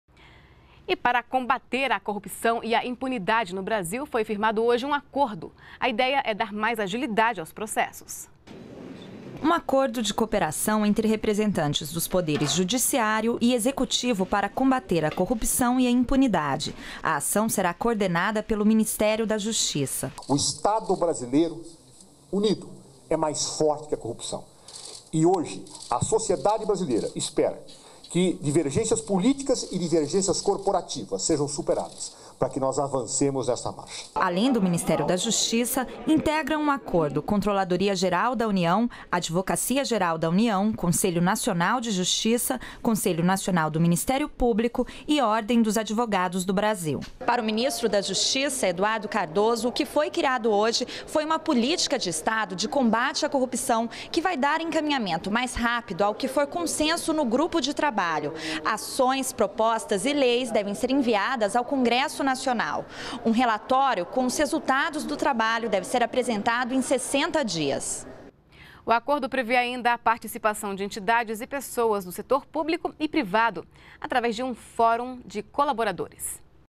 Judiciário e Executivo firmam acordo para reforçar combate à corrupção
NBR NOTÍCIAS - 25.03.15: Representantes dos poderes Judiciário e Executivo firmaram, nesta quarta-feira (25), um acordo de cooperação para combater a corrupção e a impunidade no Brasil. O objetivo do grupo é elaborar e incentivar as medidas para agilizar processos judiciais e administrativos relacionados à prática de ilícitos contra o patrimônio público. O grupo é composto pelo Ministério da Justiça, CNJ, Conselho Nacional do Ministério Público (CNMP), Advocacia-Geral da União (AGU), Controladoria-Geral da União (CGU) e Ordem dos Advogados do Brasil (OAB).